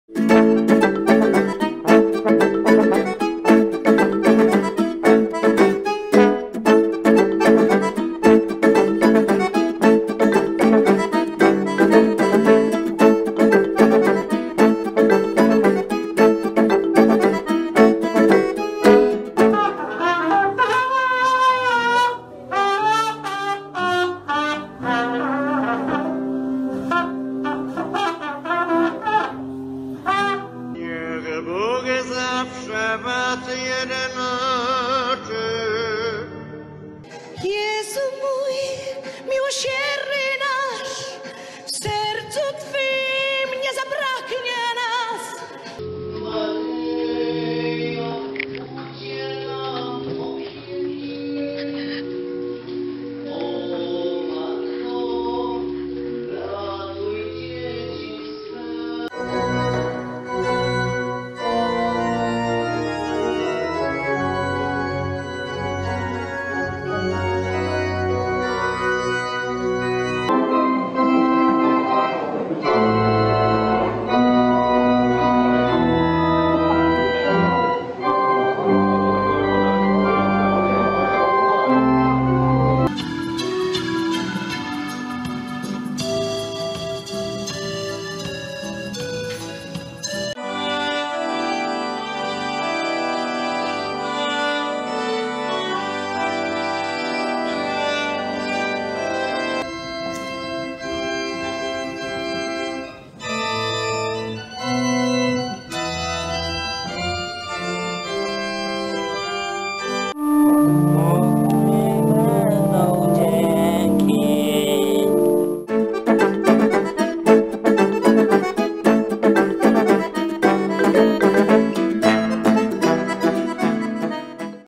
SZOK! Jak można skutecznie zrujnować swój ślub...
"Nie oszczędzaj na muzyce ani na jedzeniu dla gości.
Zespute - może przyprawić o mdłości"
GoodMood Music

Film nie ma na celu krytykowania kogokolwiek, a jedynie ostrzeżenie przed przypadkowym wyborem oprawy muzycznej na ślub. Odłożona na ostatnią chwilę może zepsuć najpiękniejszą uroczystość.
Zanim zamówisz organistę i skrzypka - koniecznie sprawdź jak grają!

Polecamy naszą profesjonalną oprawę muzyczną. Posłuchaj jak gramy klikając na filmie.

źródło: YouTube